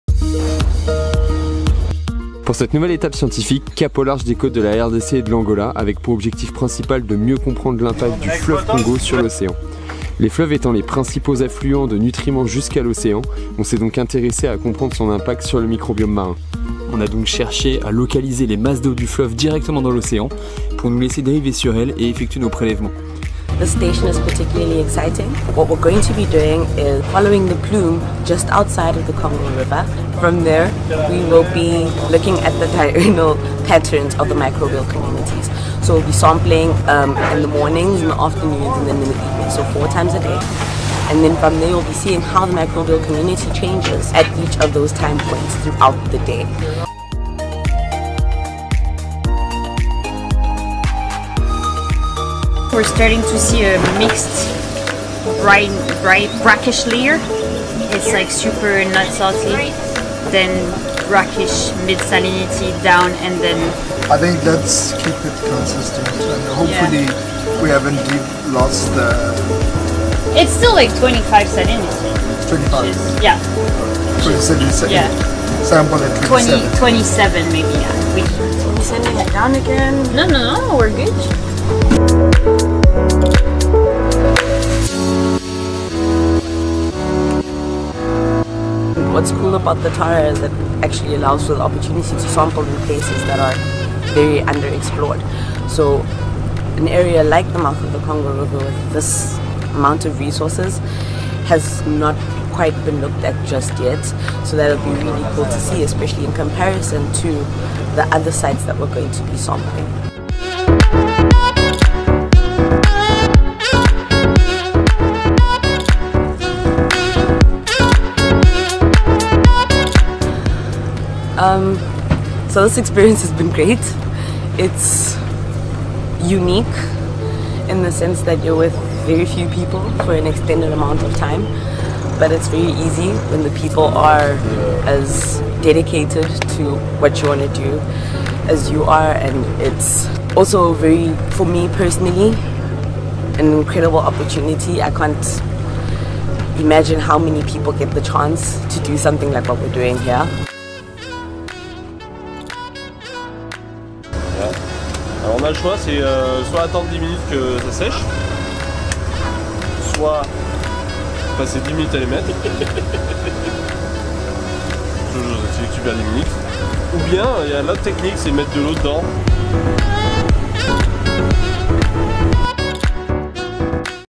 Le fleuve Congo - République Démocratique du Congo et Angola | Mission Microbiomes
⛵️ Cap au large des côtes de République Démocratique du Congo et de l’Angola à bord de la goélette Tara.

🎯 L’objectif de ces dernières semaines ? Mieux comprendre l’impact du fleuve Congo sur l’Océan et particulièrement sur le microbiome marin !

Le fleuve Congo est le deuxième plus long fleuve d’Afrique (4 700 km) derrière le Nil et le deuxième du monde pour son débit (40 000 m 3/s environ). Les fleuves étant les principaux affluents de nutriments jusqu’à l’Océan, il est important de les étudier.

📸 Guillaume Tauran // Fondation Tara Océan